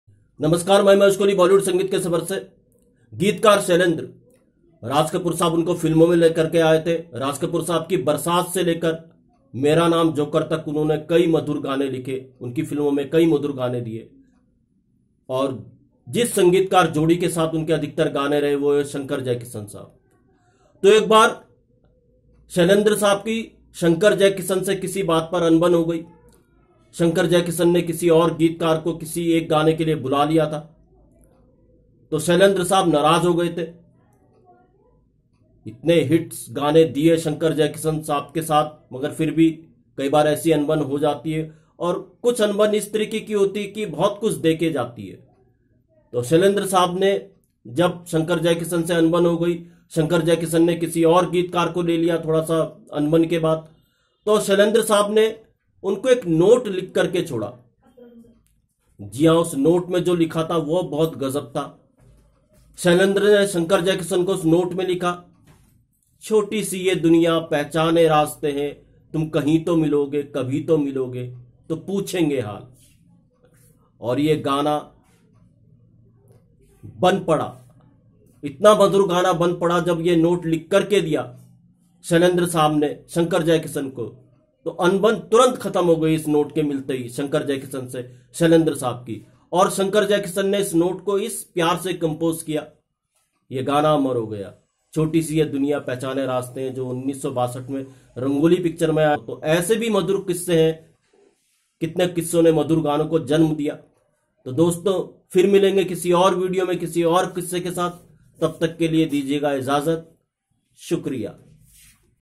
शंकर जयकिशन और शैलेन्द्र और वो अनबन कुछ इस तरह इस गीत ने जन्म लिया।
शंकर जयकिशन और शैलेन्द्र और वो अनबन कुछ इस तरह इस गीत ने जन्म लिया।@NaaradTV